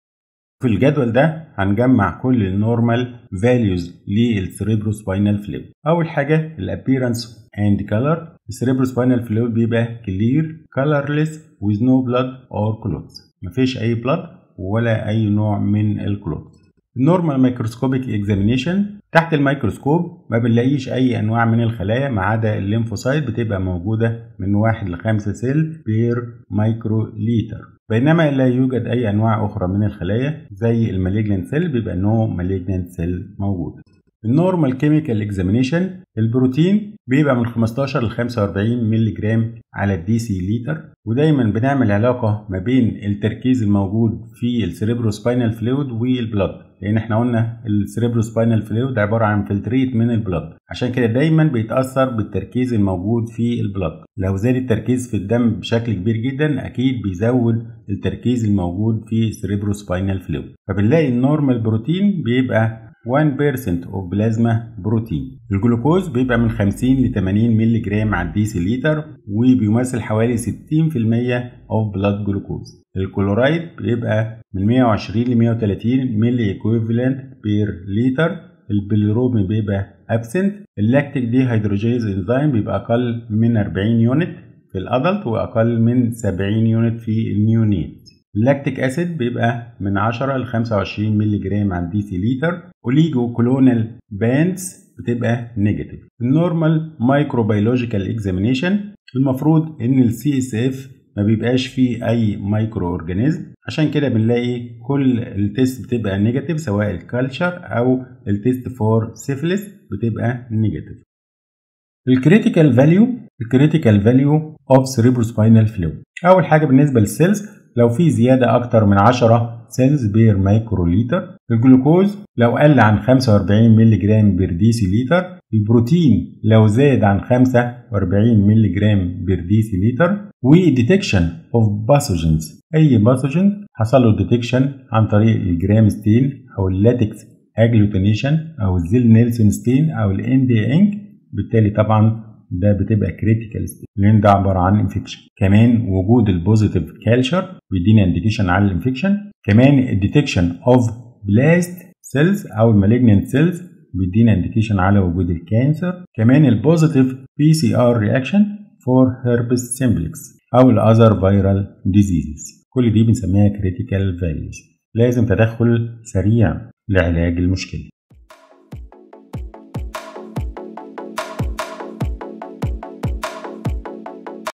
CSF. Normal and Critical Values.
Video contains:
1. Normal values of CSF analysis
2. Critical values of CSF which requires immediate intervention.